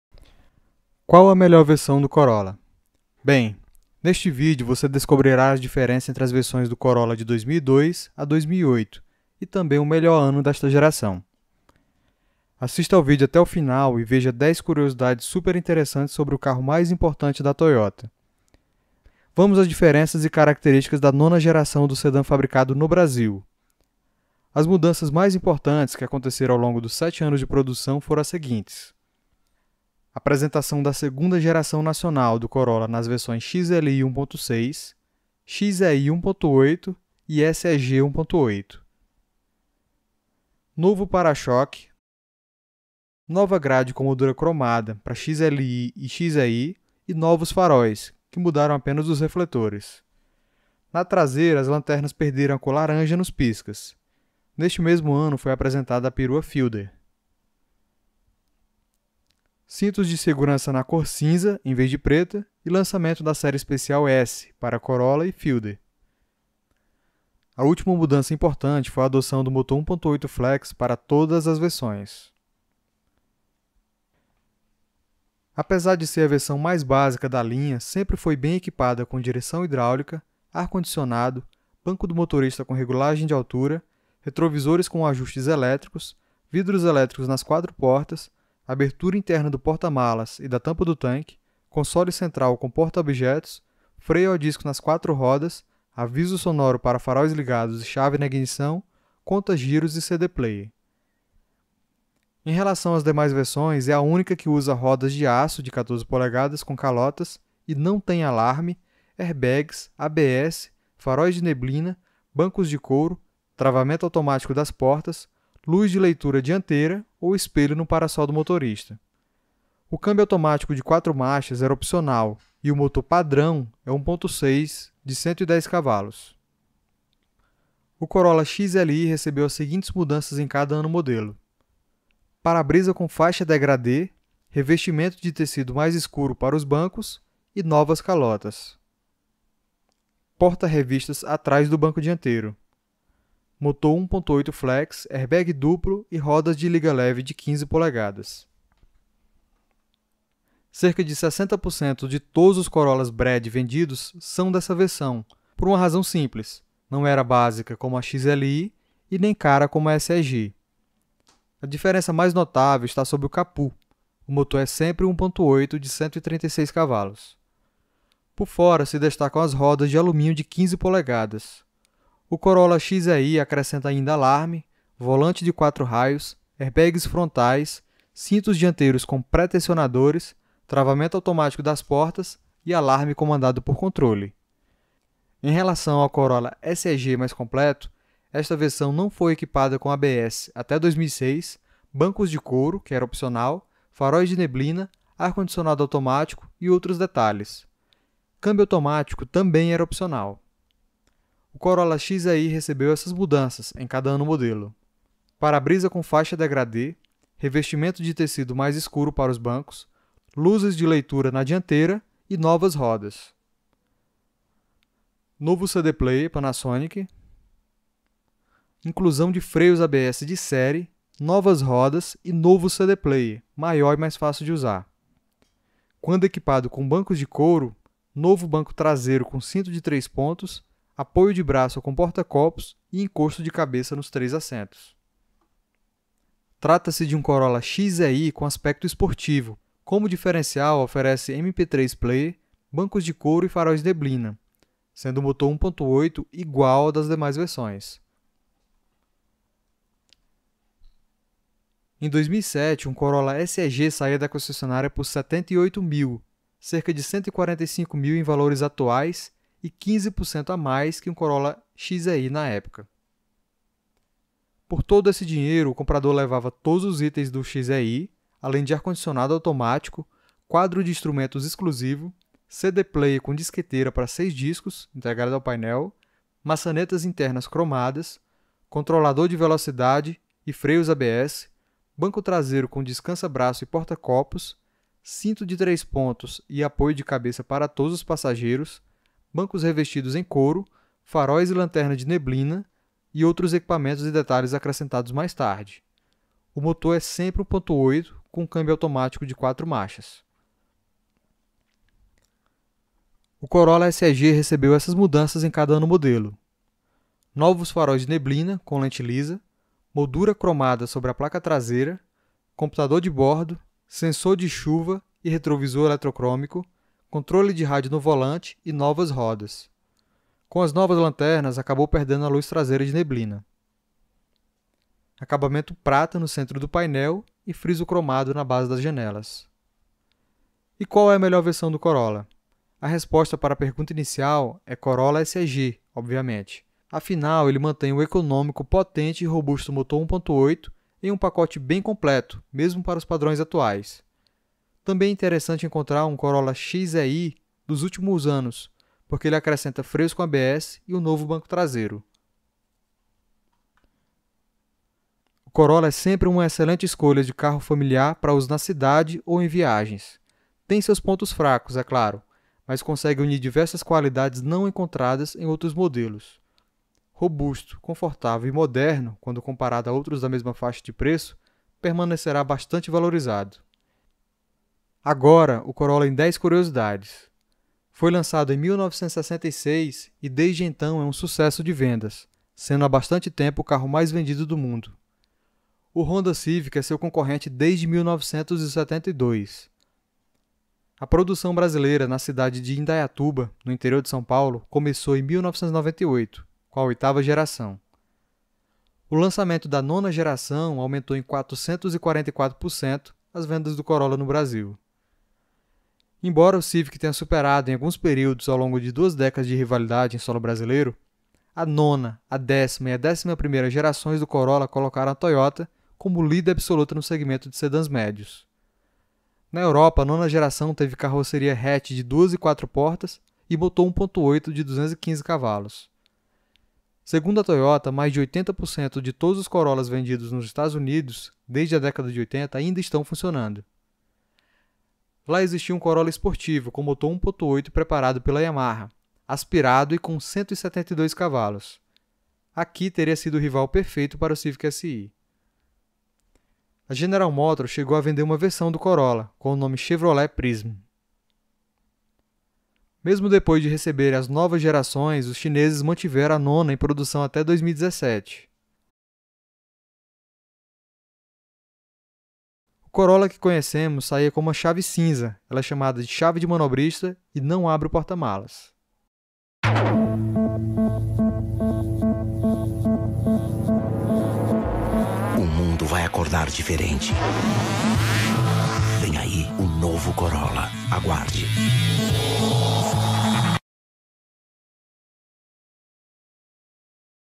Evolução do Corolla - Curiosidades!!!
É um dos carros mais queridos e vendidos no país. Veja como ele cresceu!!!